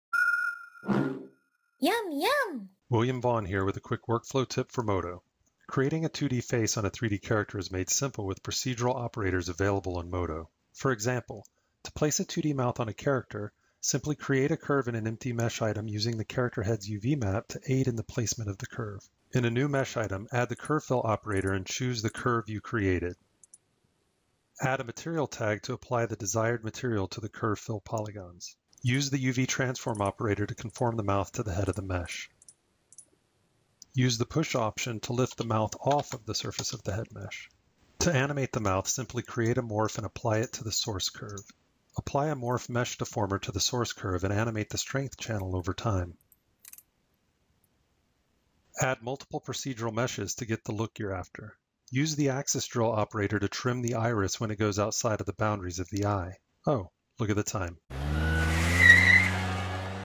MODO | 2D Face on a 3D Character
Creating a 2D face on a 3d character is made simple with procedural operators available in MODO 10.2. This video walks you through the process.